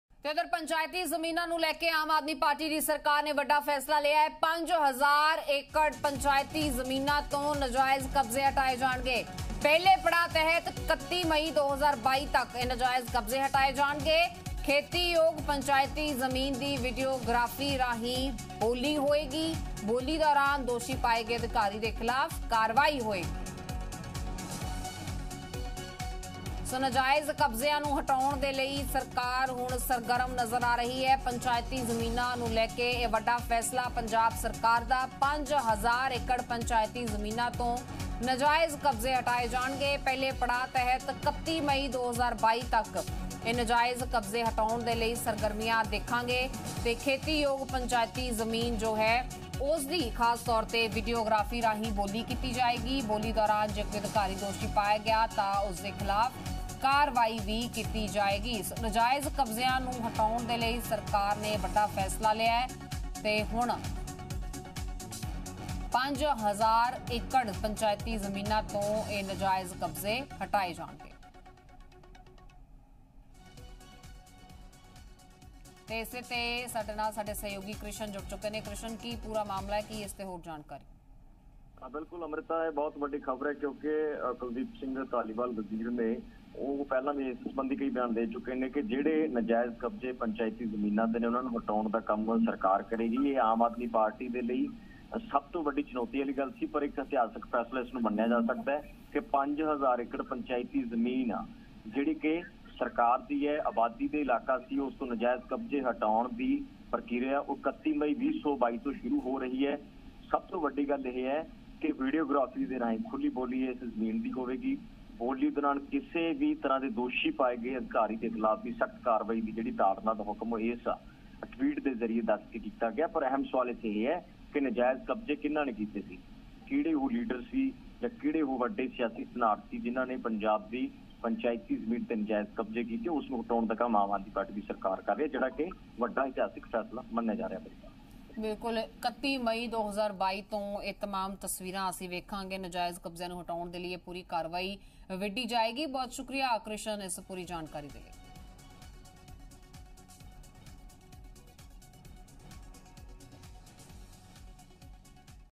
Panchayat Lands ਨੂੰ ਲੈ ਕੇ AAP ਸਰਕਾਰ ਦਾ ਵੱਡਾ ਫੈਸਲਾ, ਹਟਾਏ ਜਾਣਗੇ ਨਜਾਇਜ਼ ਕਬਜ਼ੇ | Punjab News | Zee PHH
ਪੰਚਾਇਤੀ ਜ਼ਮੀਨਾਂ ਨੂੰ ਲੈ ਕੇ 'ਆਪ' ਸਰਕਾਰ ਦਾ ਵੱਡਾ ਫੈਸਲਾ, ਹਟਾਏ ਜਾਣਗੇ ਨਜਾਇਜ਼ ਕਬਜ਼ੇ